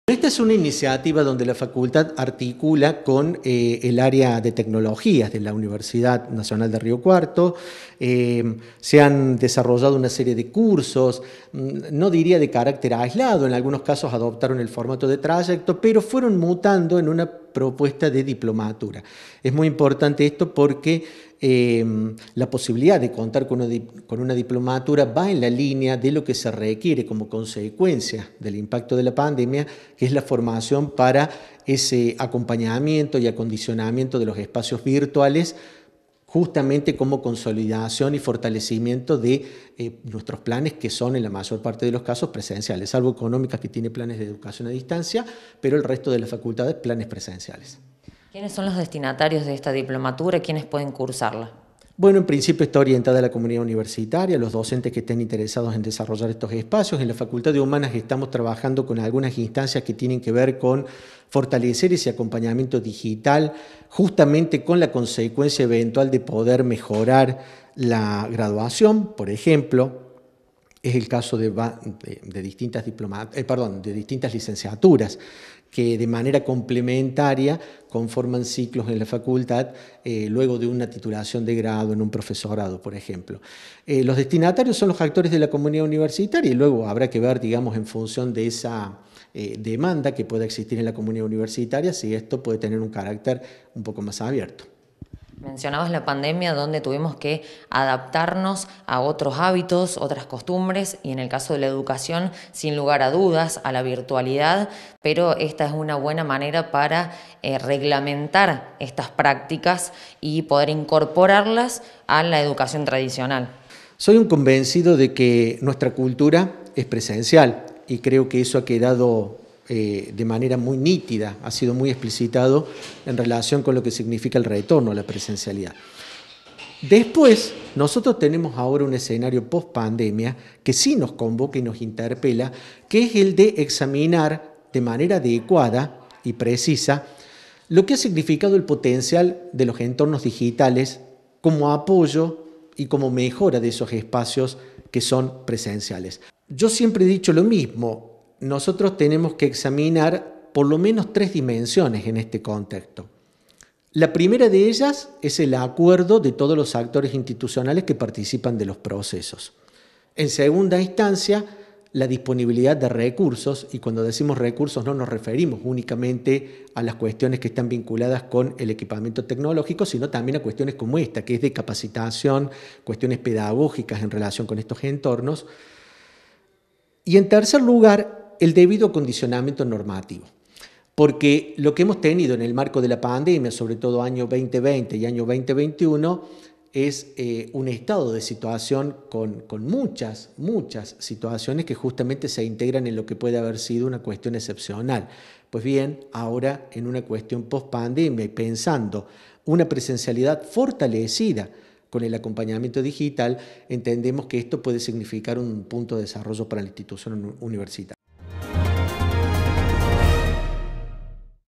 LA FACULTAD OFRECE UNA DIPLOMATURA SOBRE ENSEÑAR Y APRENDER EN ENTORNOS VIRTUALES
FABIO DANDREA – DECANO CIENCIAS HUMANAS